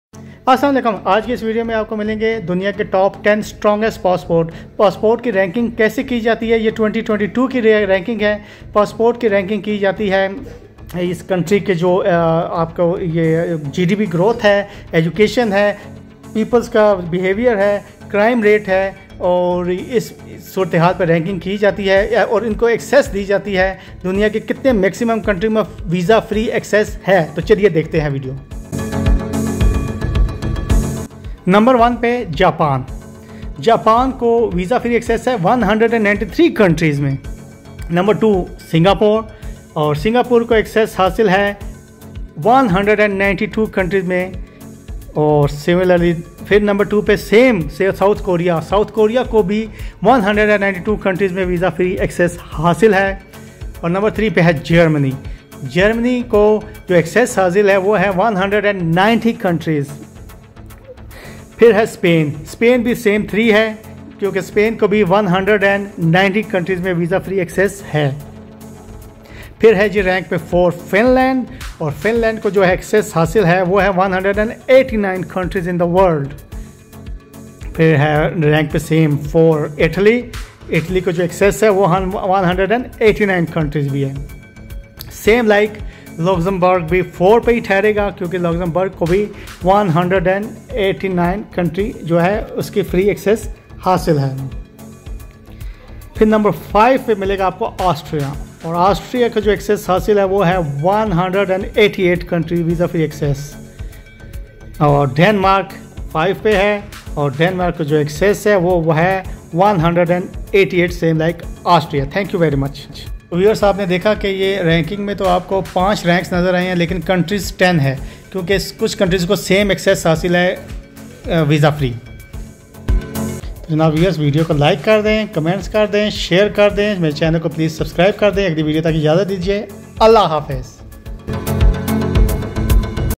Top ten passport | world strongest passports 2022 | passports ranking 2022 | number one passport
this video contains information about world top passports in 2022 list